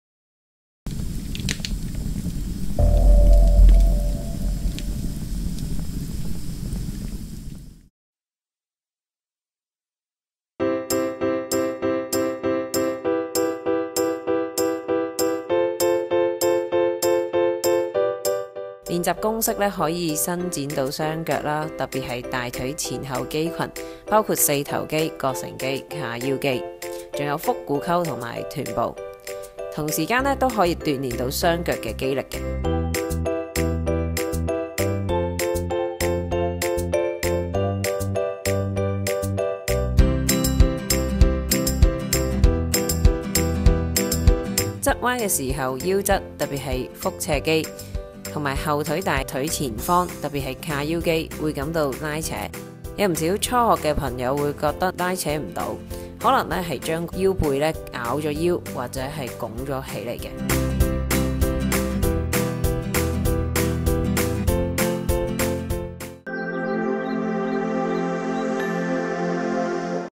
【長跑無傷 — 瑜＋跑步】第四集
跑步，一種高強度運動，要好好鍛鍊先可以跑出好成績。鍛鍊唔止係里數方面，肌肉訓練、舒緩運動亦唔可以忽視。

今次，有香港中文大學醫學院內科科學哲學博士 劉凱琳博士 示範用瑜珈動作，紓緩大腿肌群、腹股溝、臀部，想知呢個瑜伽動作點做？快啲click入嚟睇喇！

為各位跑手今年不同賽事作賽前準備，同你加油打氣！